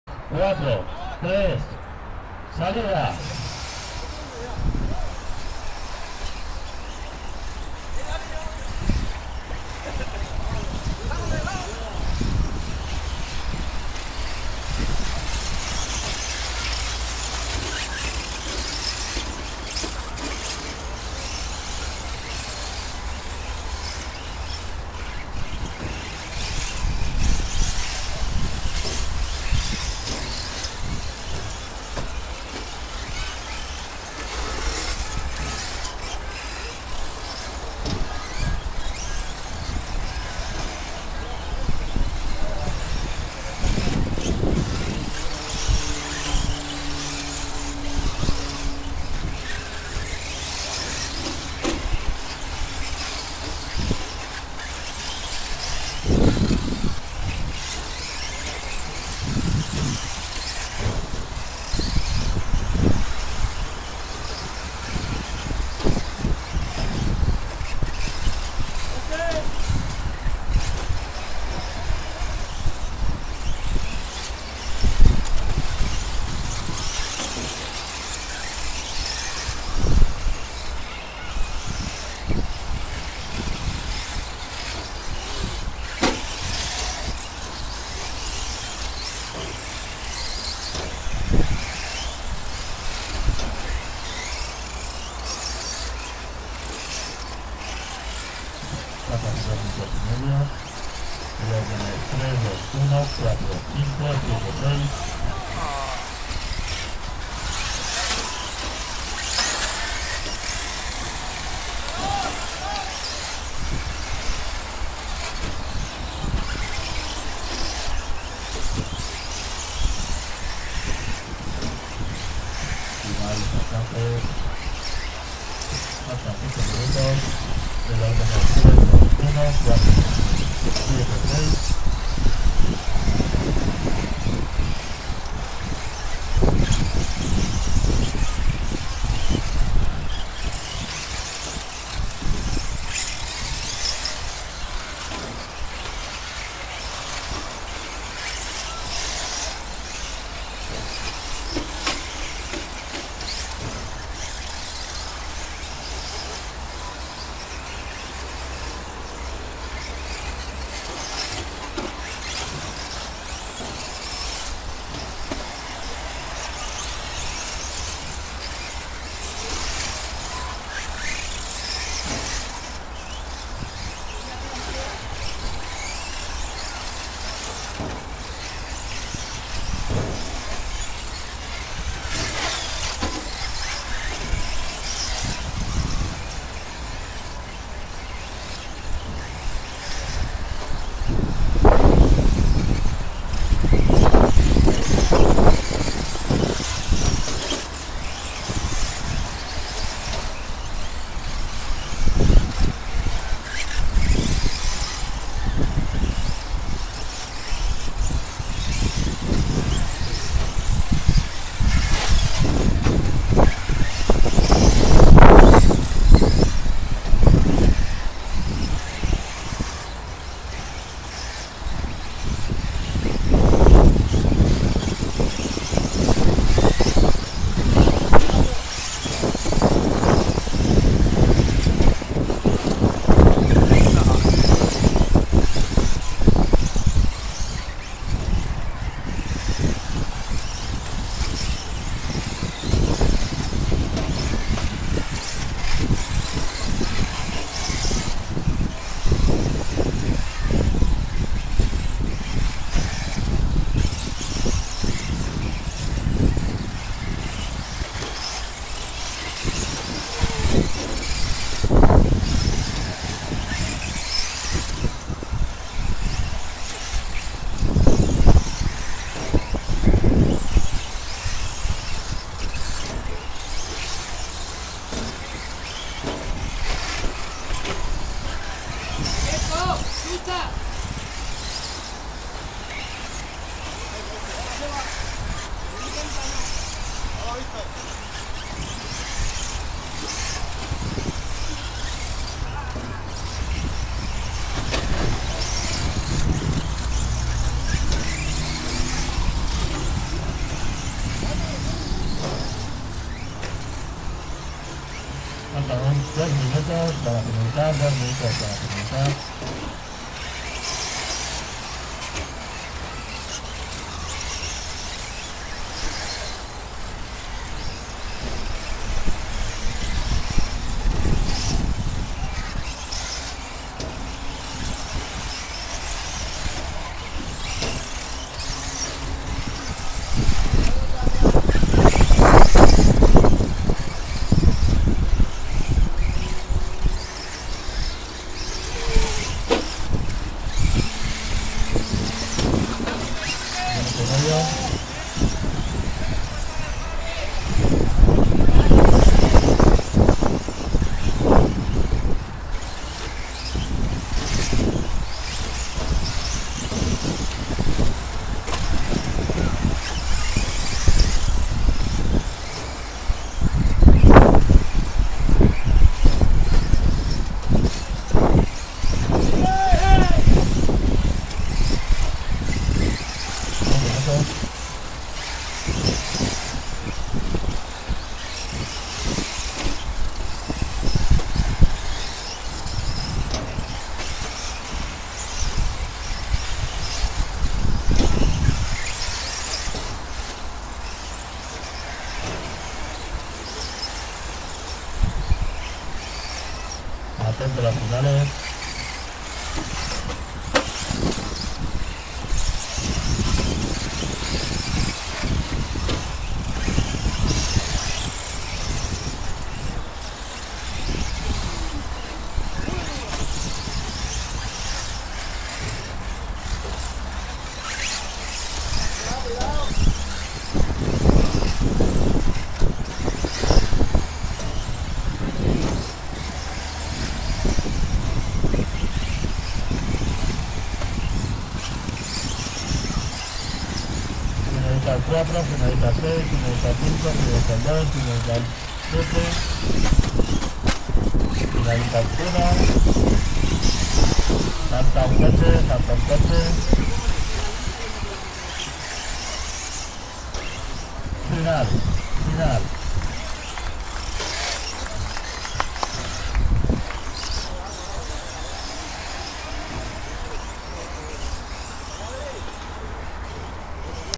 Carrera SC AMSA 03-12-17 / SEGUNDA FINAL 4x4
Ultima carrera de la temporada de la shortcourse experience 2017. En el circuito de amsa trinitat. GRABADA POR MI EN EL PODIO.